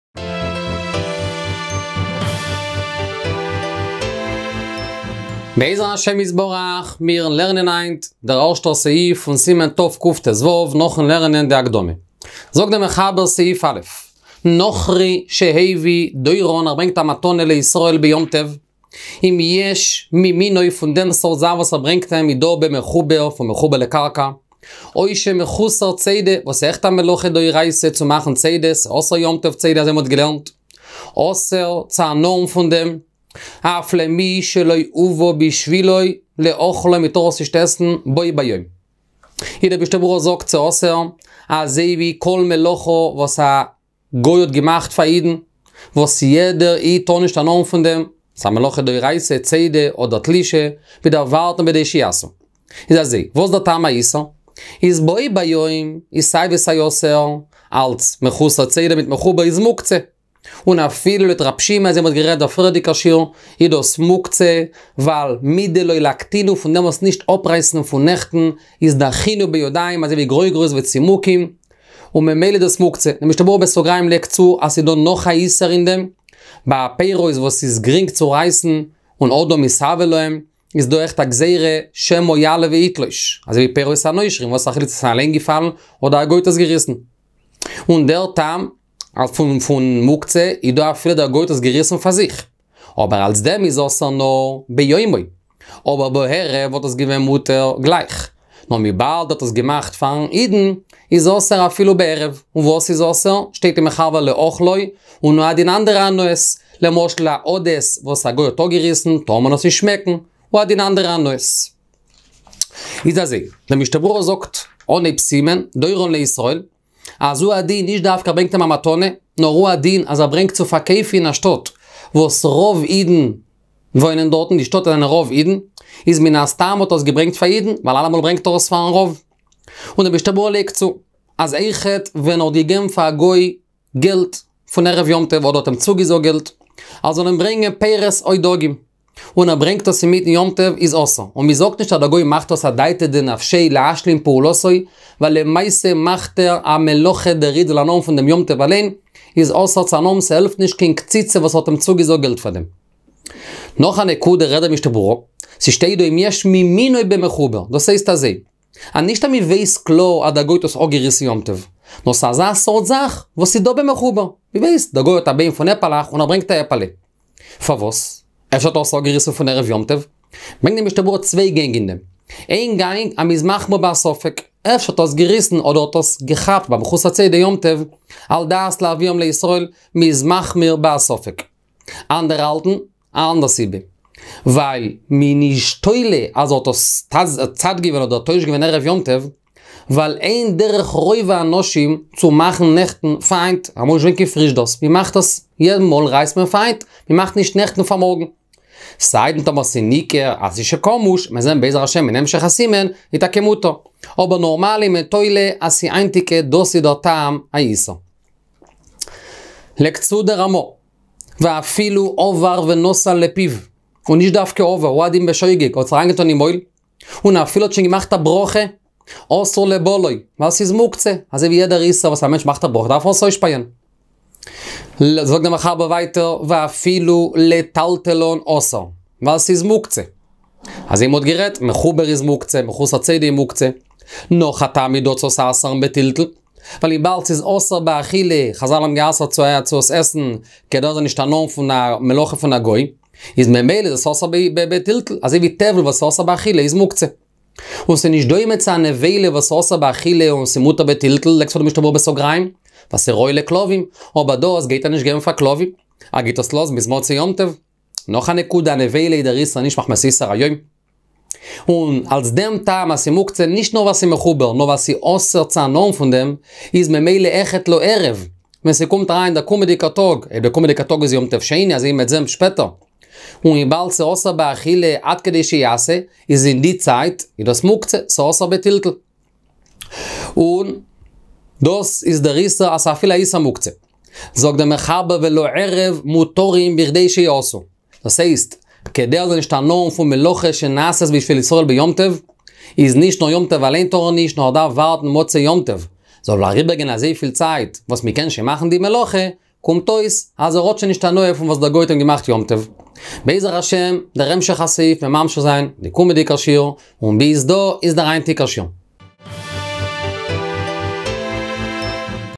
מותר לחולה להשתמש בעשבי מרפא האסורים באכילה מדרבנן?- סימן תקט"ו- אמ' סע' אמצע "ובשני יו"ט של גלויות"
הדף היומי באידיש מבית "דרשו" עם הרב אפרים סגל שליט"א